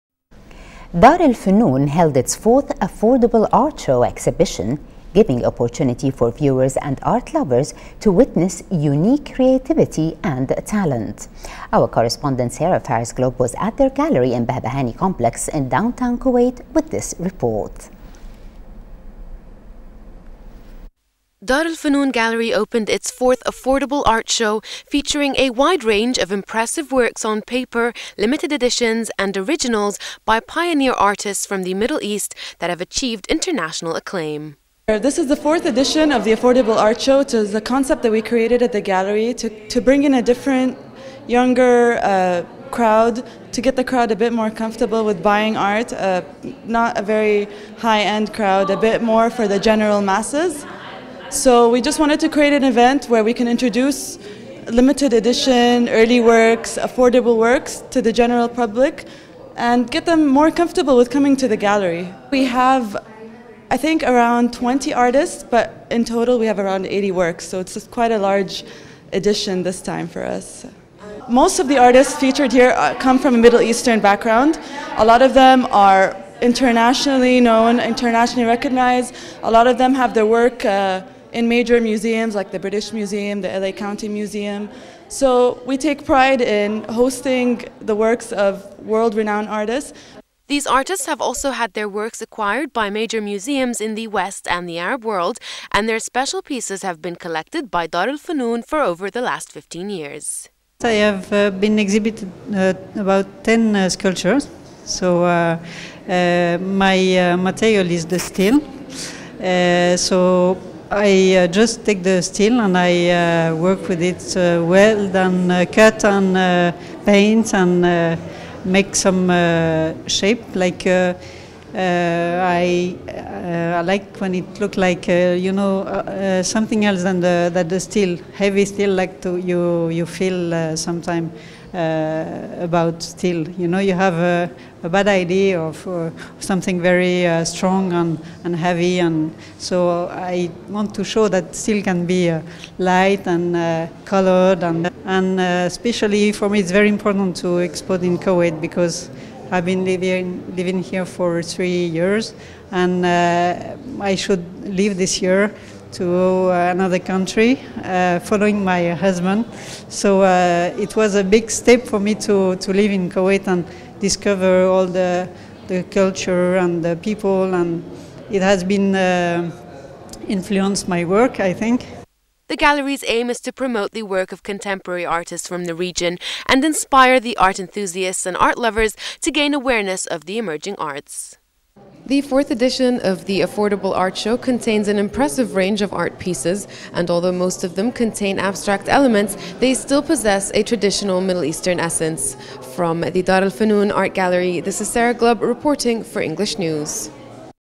Dar Al-Funoon holds 4th Affordable Art Show Exhibition
Dar Al Funoon held its 4th Affordable Art Show exhibition, giving opportunity for viewers and art lovers to witness unique creativity and talent.